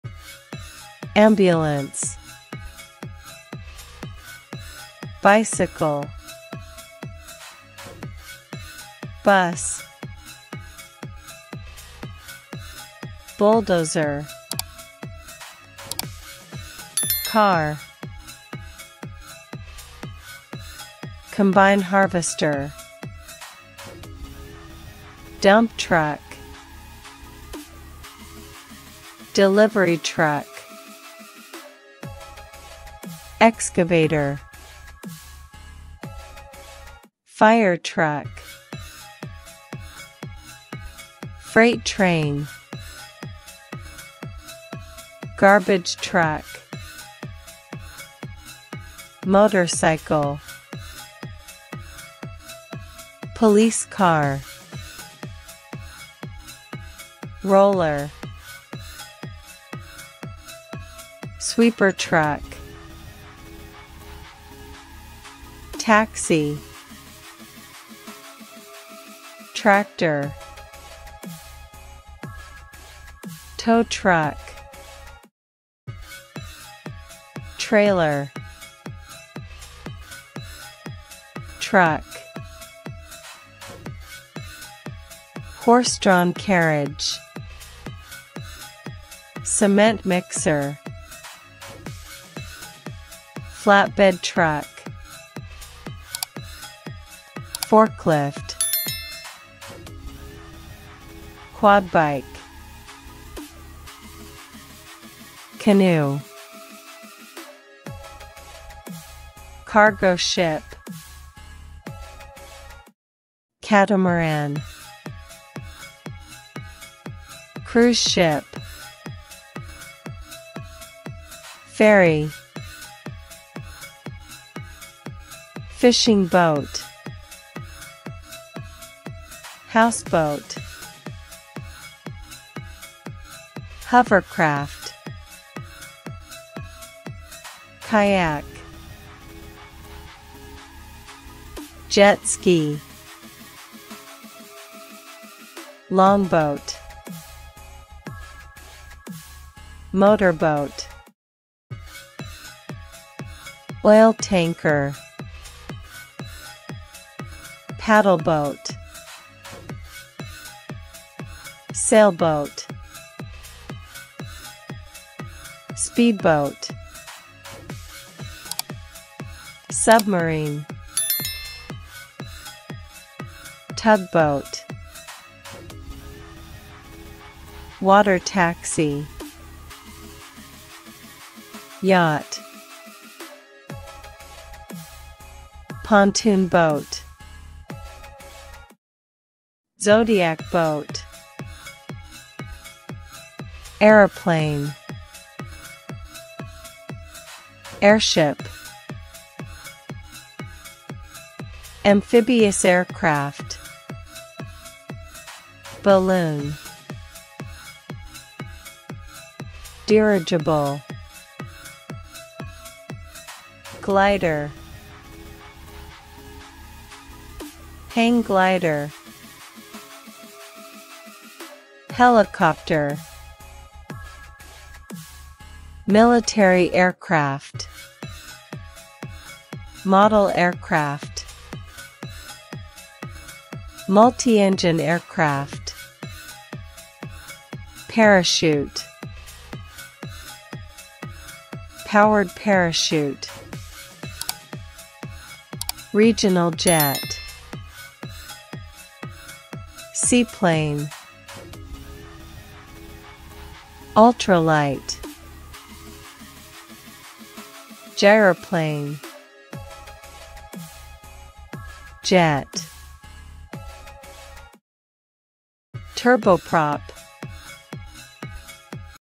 Vehicles Names In English With Pictures | Learn Vehicles For Kids
Welcome to our exciting video.
 In this video, we'll be exploring various categories of vehicles, including street, emergency, construction, water, air, and rail transport.